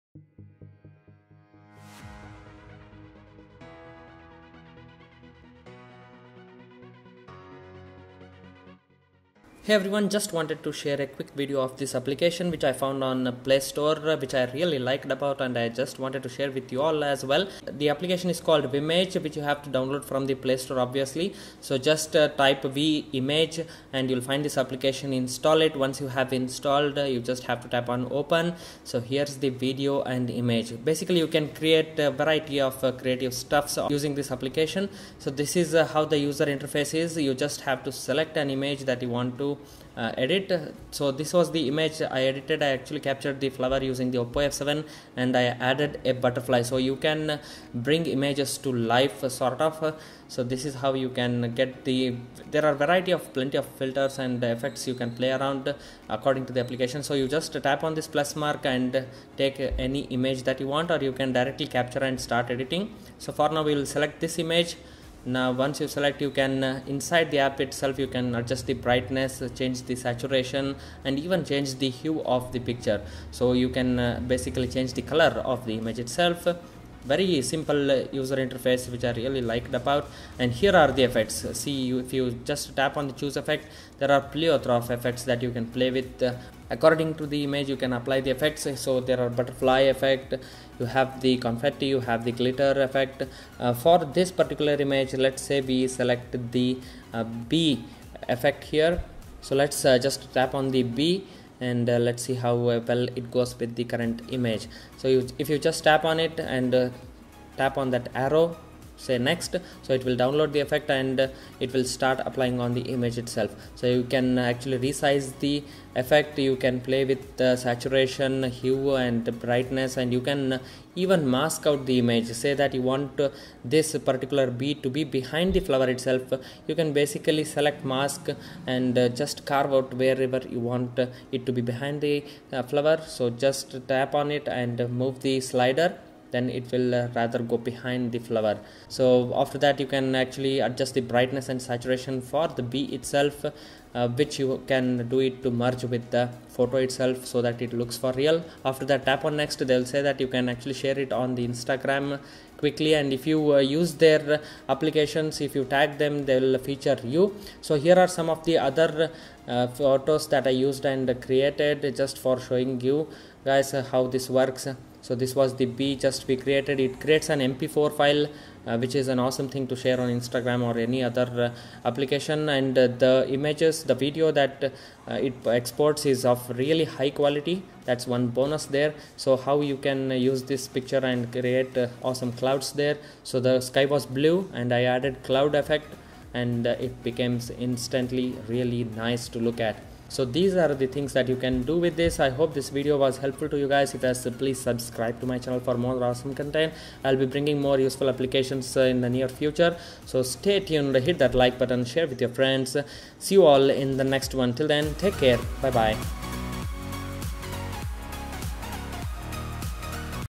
Bring Images to Life with this APP | Screen Recorded using Oppo F7
Vimage app tutorial video is recorded using Oppo F7 Screen Recorder.

Vimage app is basically a Video Imaging Application available on Android using which you can create mp4 videos of any Images/pictures while adding awesome effects.

So many beautiful effects can be added to a image to make it look creative and awesome !

PS: Nobody endorsed me to make this app tutorial. I am not paid by the app owners. I personally enjoyed using this application and I am sharing with my audience hoping it helps them create new stuff :)